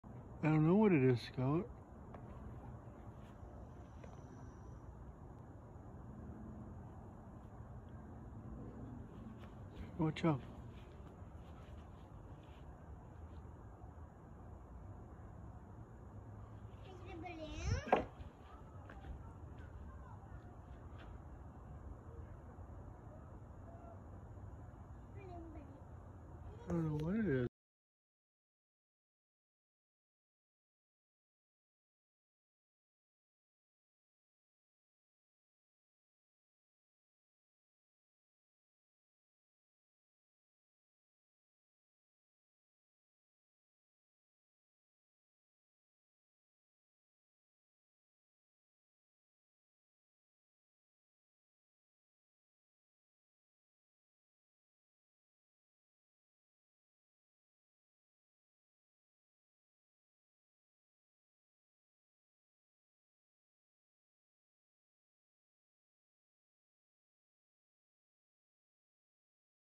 Des Moines WA UFO UAP 6/13/20
Saw this while I was out in the back yard, used basic contrast and zoom options on my IPhone to try and clean it up a little. Look bigger in person than the video shows and there was no sound coming from it. I saw two of these object with it a few minutes and the second one was the one I caught On camera. I watched it till I could no longer see it as it moved off into the distance.